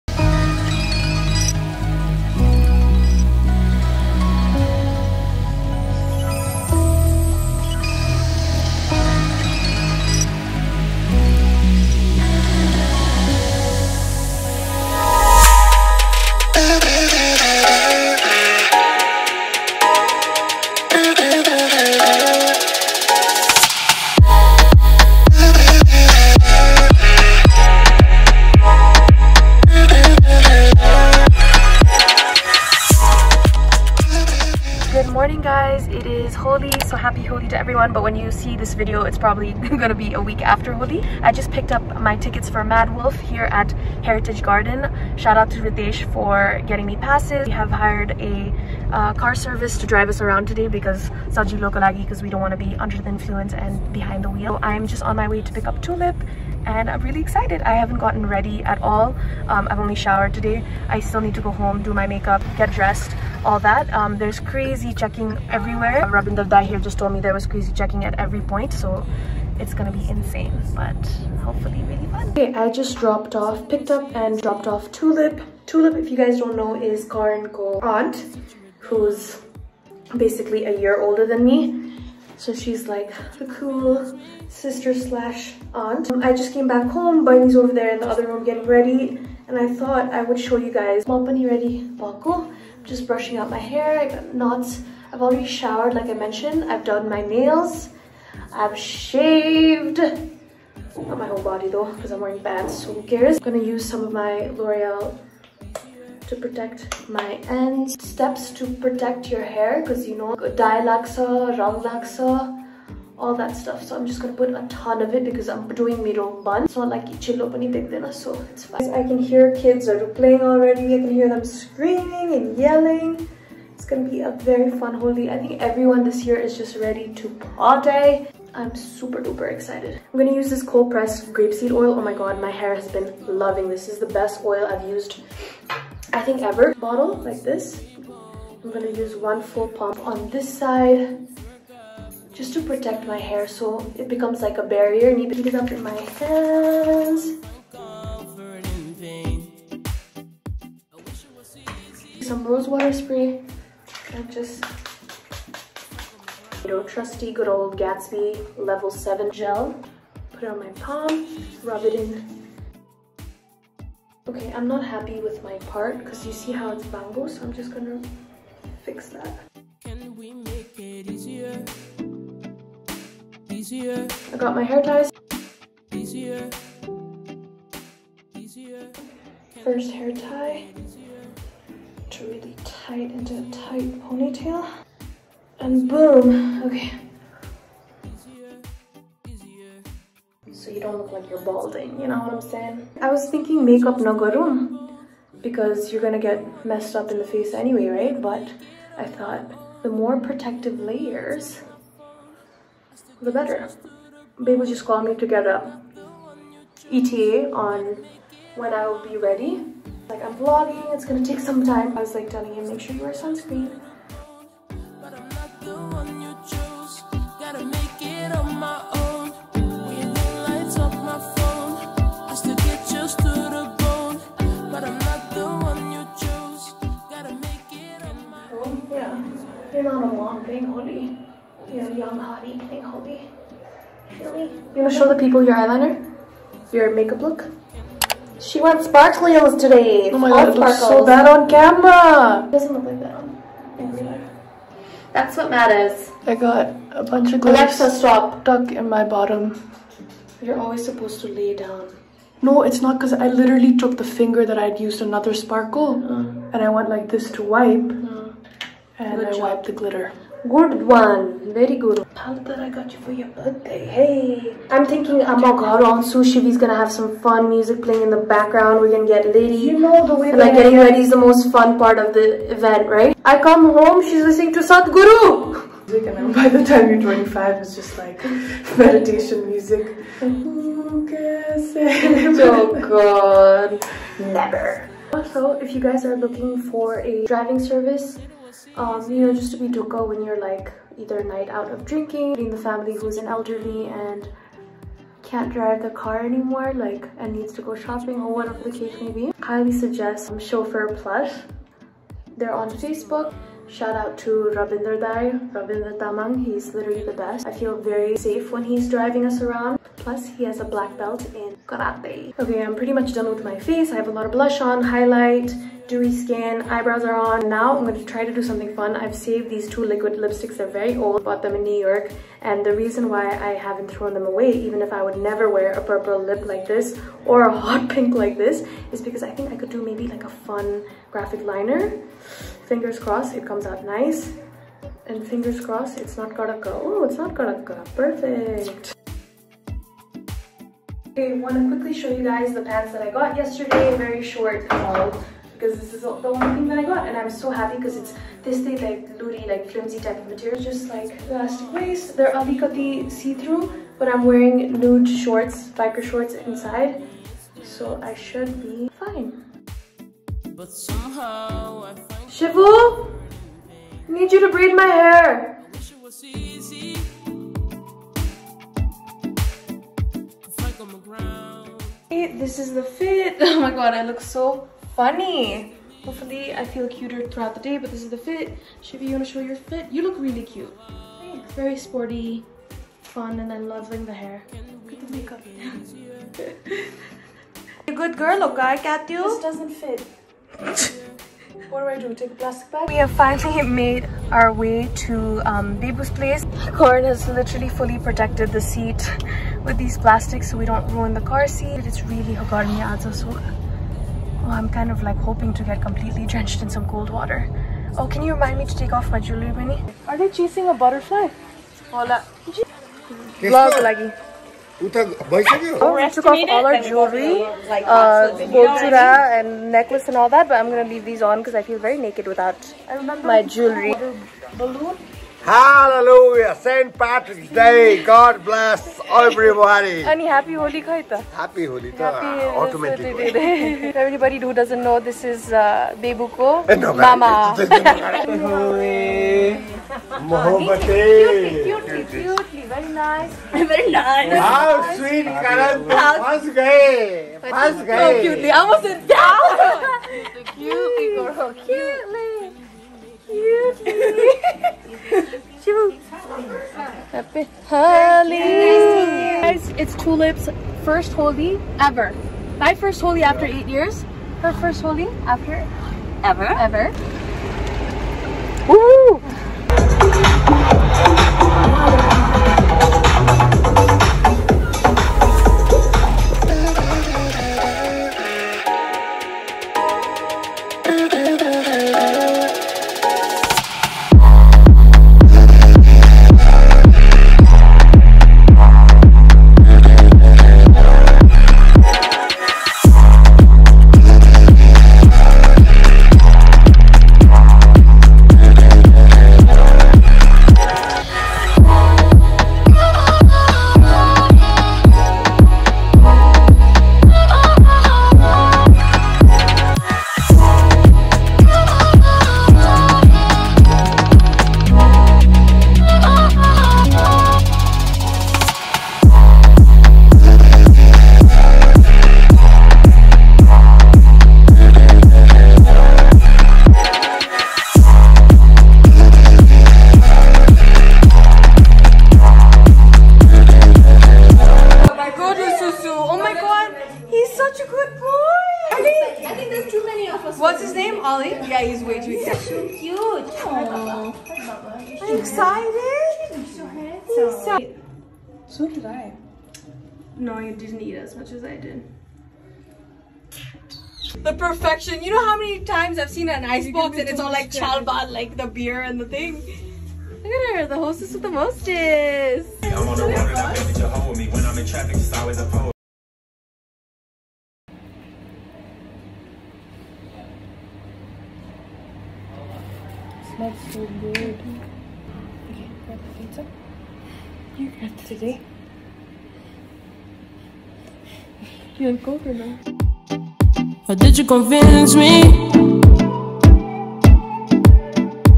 celebrating holi after years (vlog)
hiii. i managed to edit & get this video up on time. shocker there. hope everyone had a safe & fun holi! much love xx